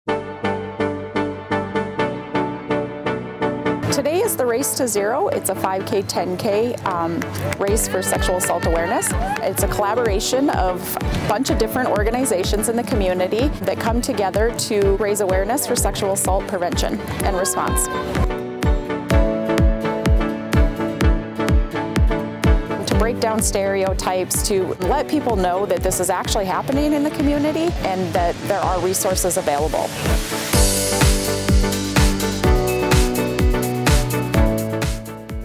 Race to Zero 2024
What a great 'Race to Zero'!  We continue to take strides towards a safer and more respectful world.
We will continue to raise awareness, support survivors, and work together to eliminate sexual assault.
Together, we can make a difference! RaceToZero EndSexualAssault SupportSurvivors 🏃‍♀️💪🏽🌟

To learn more about North Dakota National Guard Sexual Assault Prevention and Response program please visit